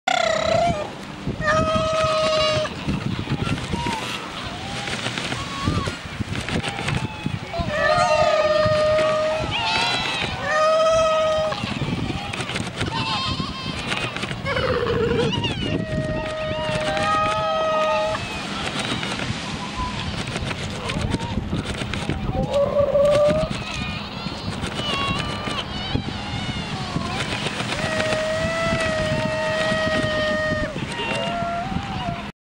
Sights and Sounds of Fortuna Bay, South Georgia Islands
Fur seals and King penguins abound on the shore at Fortuna Bay.  The video was taken on January 4, 2013, during an expedition celebrating the 125th anniversary of the Geological Society of America.